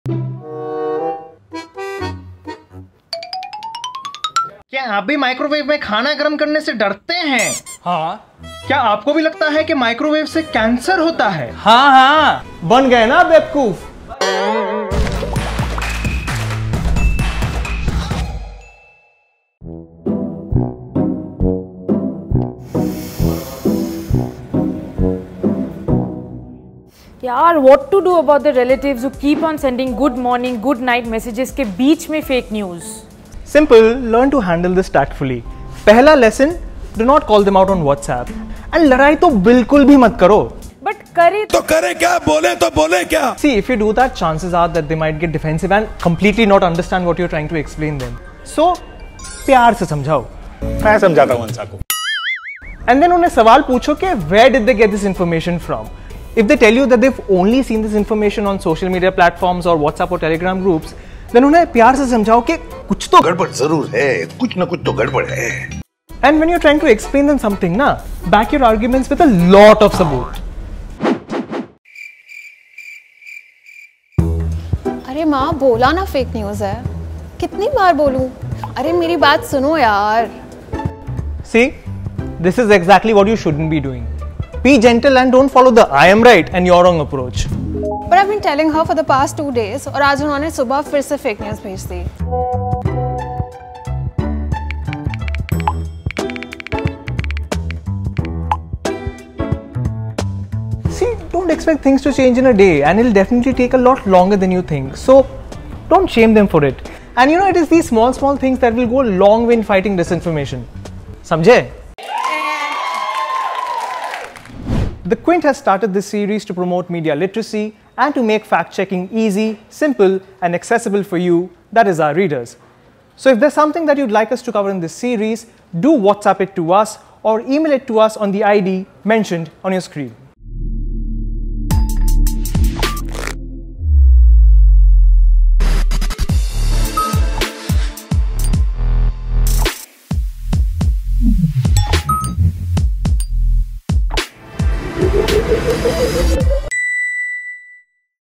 Verify Kiya Kya? | How To Talk to Your Family About the Misinformation They Keep Sharing | The Quint
While talking to your family or even friends about the misinformation they share, be empathetic, and give them time to change their behaviour. Don’t shame them or expect things to change immediately. Watch our video to know more.